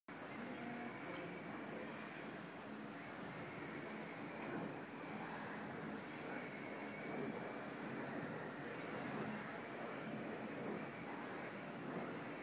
Adams Creek R/C Tulsa, OK
View of the outdoor off-road track